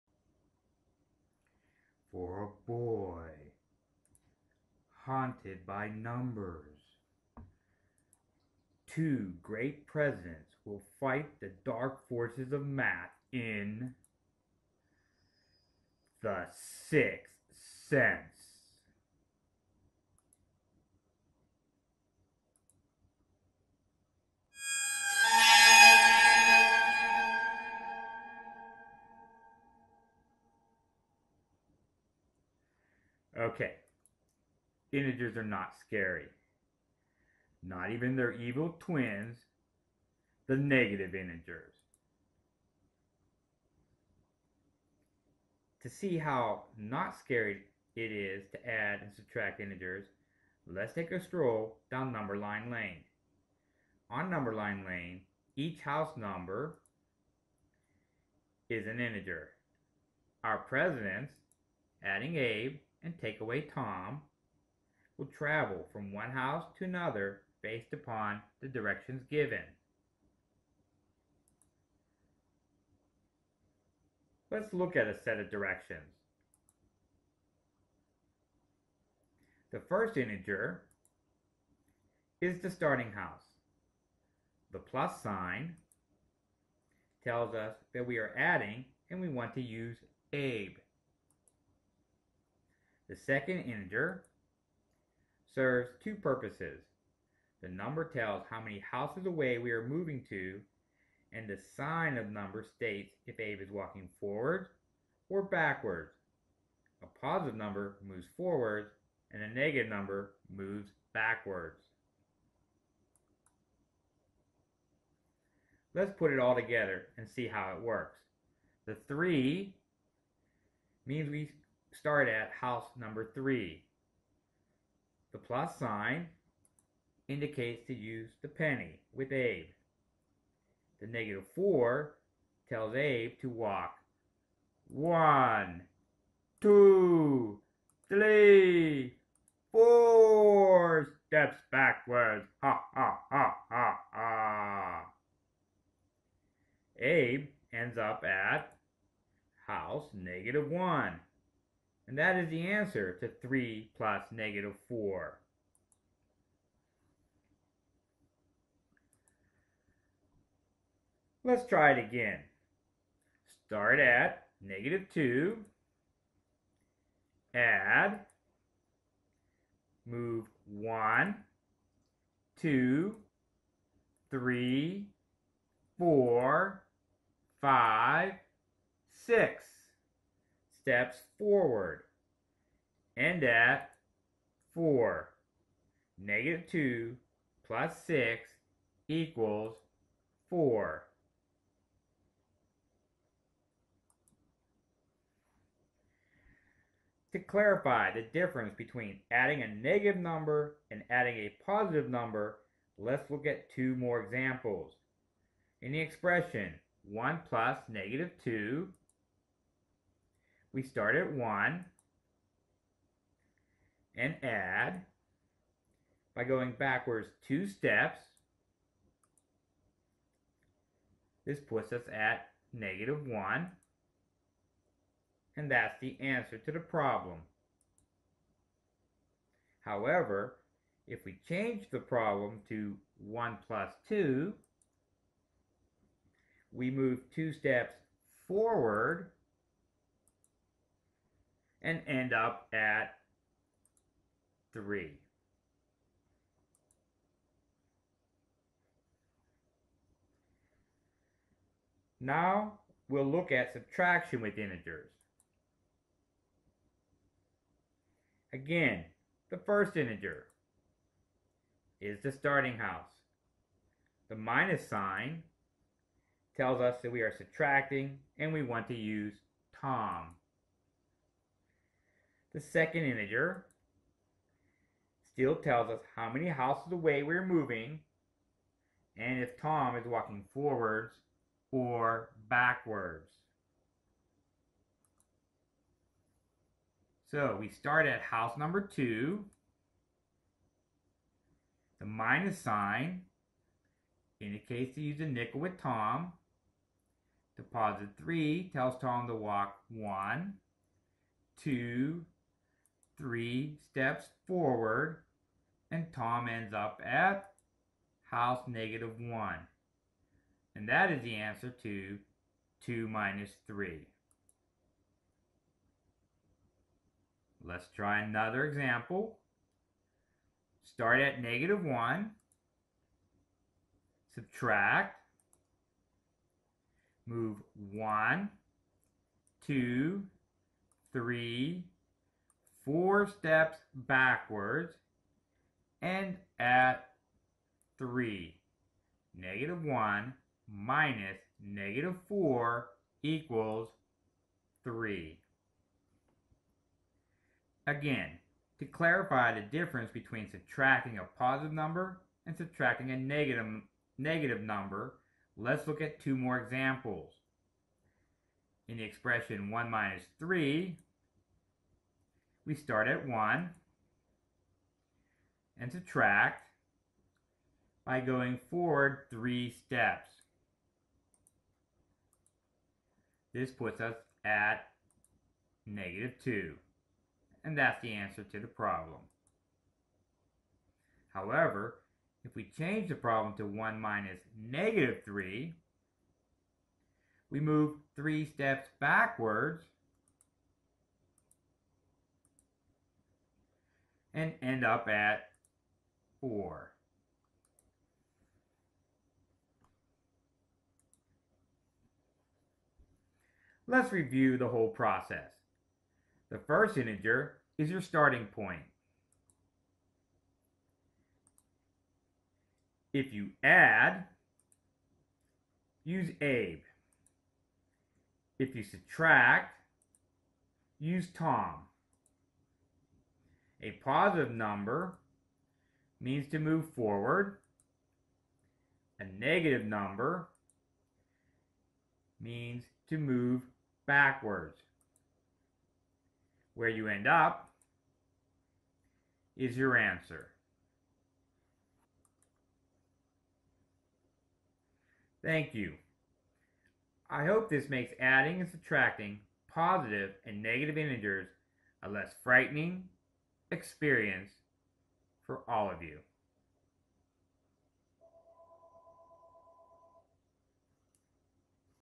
The Six Cents: How to Add and Subtract Integers
This is a PowerPoint Movie depicting a method of adding and subtracting
positive and negative integers. It is an original method. By that, I
mean I never seen any else ever publish anything similar to this method
in the 15 years or so since I first developed it. The method involves
the use of two coins, a penny and a nickel (hence, six cents) and a
number line.